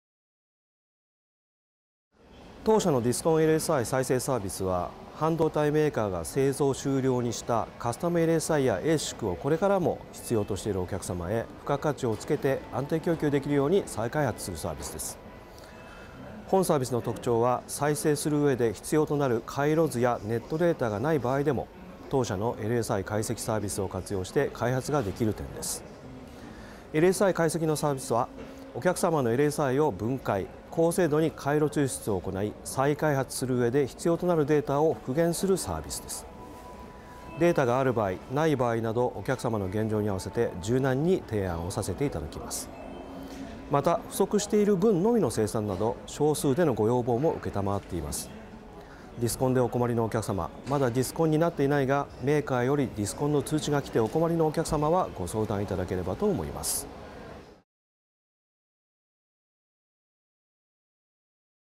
ディスコンLSI再生サービス : IIFES2019
東芝情報システム（株）は、東芝デジタルソリューションズ（株）のグループ会社です。

■ディスコンLSI再生サービス
過去に開発したLSIを、新しいプロセスで作り替えることにより継続供給するサービス

その他の東芝グループのデモンストレーション映像や会場の様子、出展製品の詳しい情報はこちらをご覧ください。